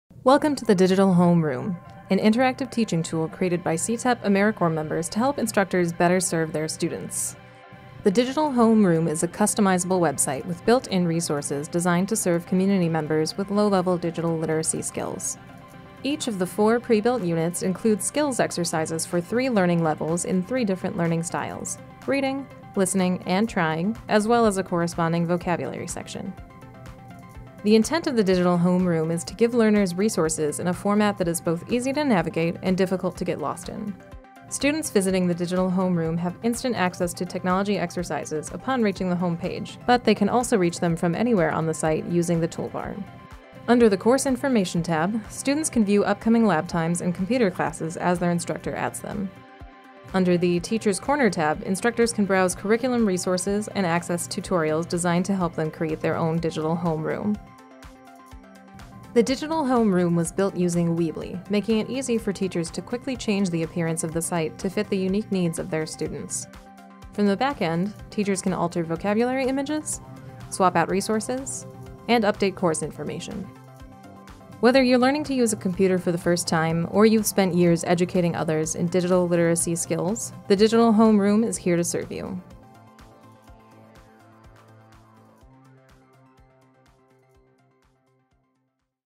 CTEP Weebly Digital Homeroom Intro Video
Learn how the CTEP Weebly page can help your beginning English language learners practice technology skills at their own pace.